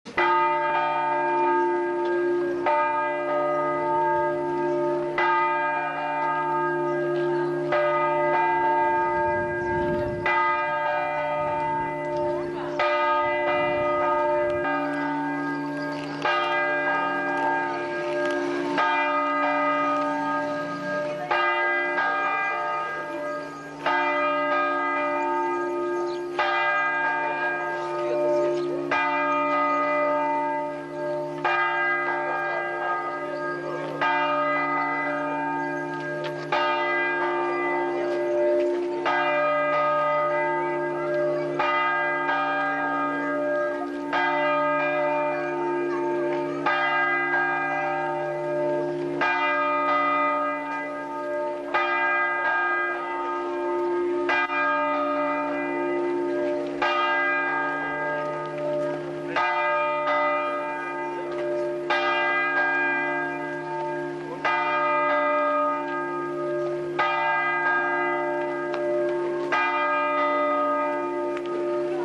Défilé du cortège de la Fête-Dieu à Ossès(64). Juin 87.
Défilé du cortège de la Fête-Dieu à Ossès(64) en Juin 1987.